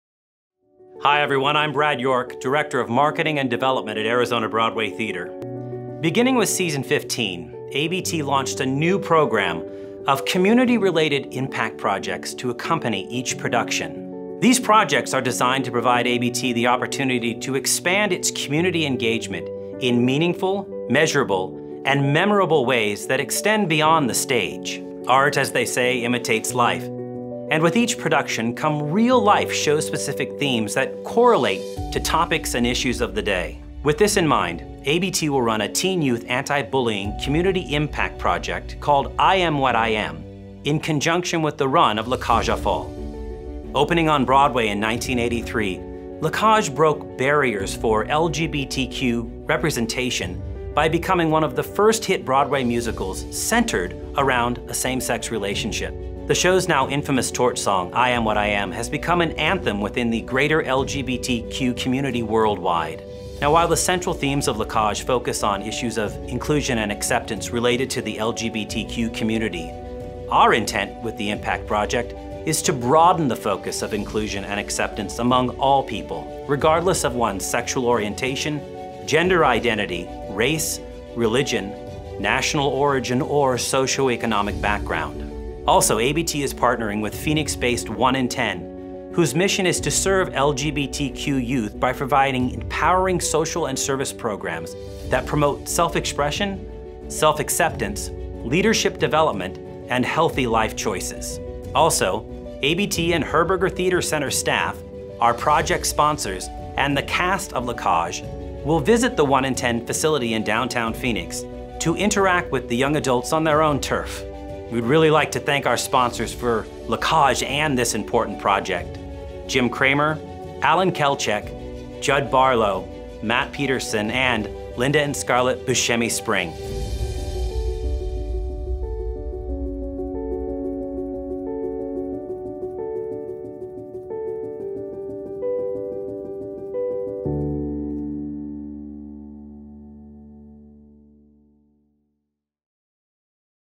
ABT Impact Anti Bullying Project
Brad York announces the Arizona Broadway Theatre Impact Project program and the Anti-Bullying Campaign during the run of La Cage aux Folles